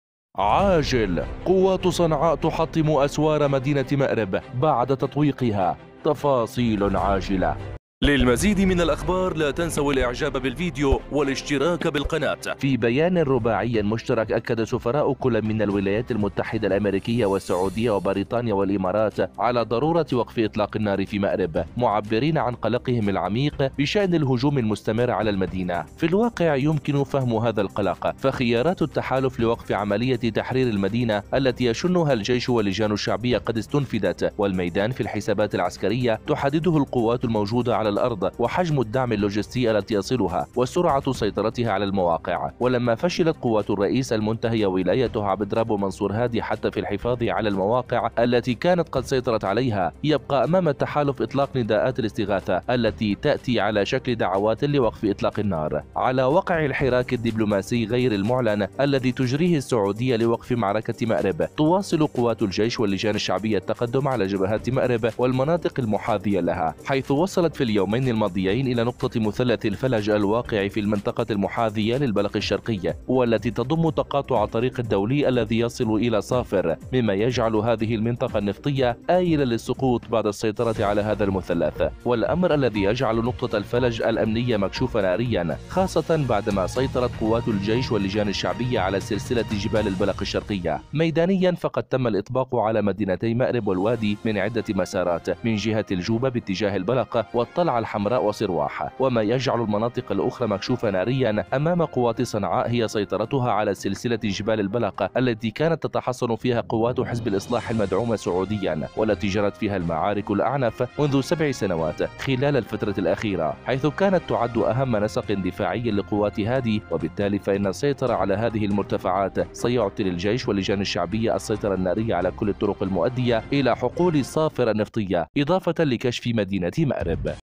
الله أكبر💥الحوثيين تحط،م بوابه مأرب وتنتصر واجتماع سعودي بريطاني اماراتي عاجل !!
الله أكبر💥الحوثيين تحطم بوابه مأرب وتنتصر واجتماع سعودي بريطاني اماراتي عاجل !!
في بيان رباعي مشترك، أكد سفراء كل من الولايات المتحدة والسعودية وبريطانيا والامارات، على ضرورة وقف إطلاق النار في مأرب، معبّرين عن “قلقهم العميق بشأن الهجوم المستمر على المدينة”. في الواقع يمكن فهم هذا القلق، فخيارات التحالف لوقف عملية تحرير المدينة التي يشنها الجيش واللجان الشعبية قد استُنفذت، والميدان -في الحسابات العسكرية- تحدده القوات الموجودة على الأرض وحجم الدعم اللوجستي التي يصلها وسرعة سيطرتها على المواقع، ولما فشلت قوات الرئيس المنتهية ولايته عبد ربه منصور هادي حتى في الحفاظ على المواقع التي كانت قد سيطرة عليها، يبقى أمام التحالف إطلاق “نداءات الاستغاثة” التي تأتي على شكل دعوات لوقف إطلاق النار.
على وقع الحراك الدبلوماسي غير المعلن الذي تجريه السعودية لوقف معركة مأرب، تواصل قوات الجيش واللجان الشعبية التقدم على جبهات مأرب والمناطق المحاذية لها، حيث وصلت في اليومين الماضيين إلى نقطة مثلث الفلج الواقع في المنطقة المحاذية للبلق الشرقي والتي تضم تقاطع الطريق الدولي الذي يصل إلى صافر، مما يجعل هذه المنطقة النفطية آيلة للسقوط بعد السيطرة على هذا المثلث. والأمر الذي يجعل نقطة الفلج الأمنية مكشوفة نارياً خاصة بعدما سيطرت قوات الجيش واللجان الشعبية على سلسلة جبال البلق الشرقية.
ميدانياً، فقد تم الاطباق على مدينتي مأرب والوادي من عدة مسارات، من جهة الجوبة باتجاه البلق، والطلعة الحمراء وصرواح، وما يجعل المناطق الأخرى مكشوفة نارياً أمام قوات صنعاء هي سيطرتها على سلسلة جبال البلق التي كانت تتحصن فيها قوات حزب الإصلاح المدعومة سعودياً، والتي جرت فيها المعارك الأعنف منذ 7 سنوات خلال الفترة الأخيرة، حيث كانت تعد أهم نسق دفاعي لقوات هادي، وبالتالي فإن السيطرة على هذه المرتفعات سيعطي للجيش اللجان الشعبية السيطرة النارية على كل الطرق المؤدية إلى حقول صافر النفطية، إضافة لكشف مدينة مأرب